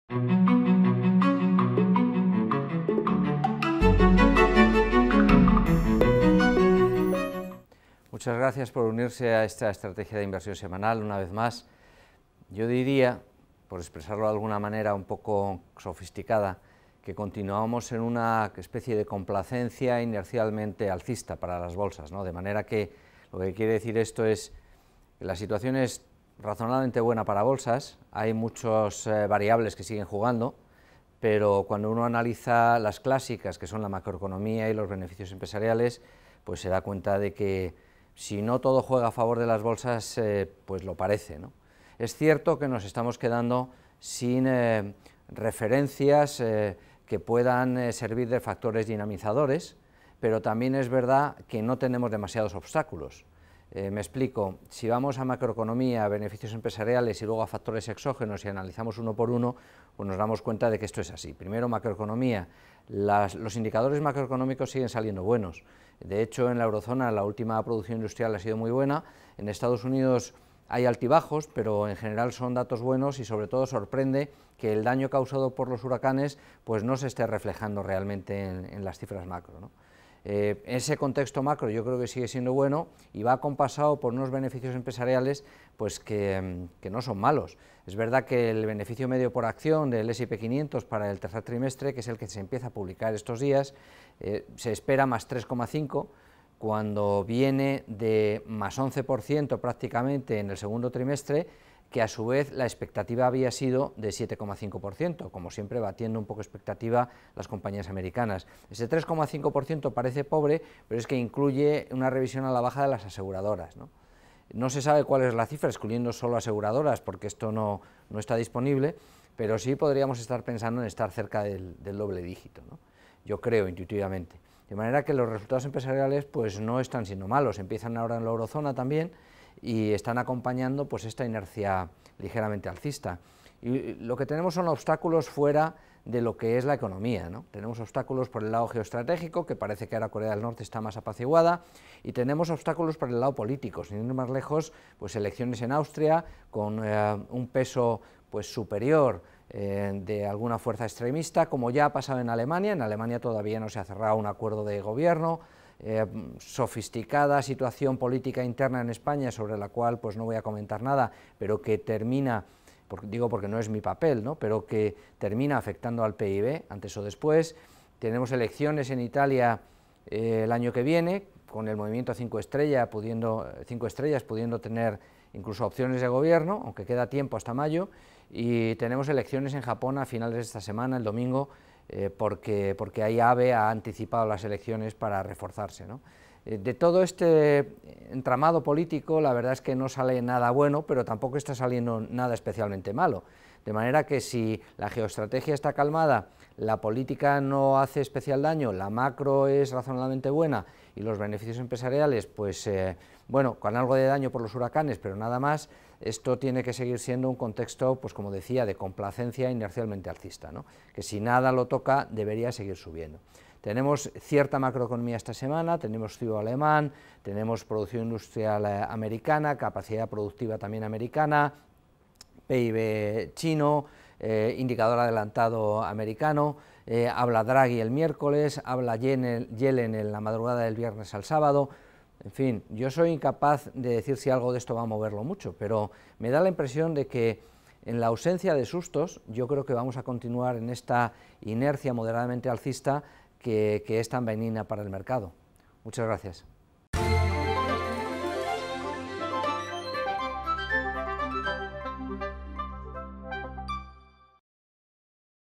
¿Qué pasará la próxima semana en los mercados? (16-20 de octubre)
¿Qué pasará la próxima semana en los mercados?
Ramón Forcada, Director de Análisis de Bankinter, nos explica las claves que moverán los mercados la próxima semana.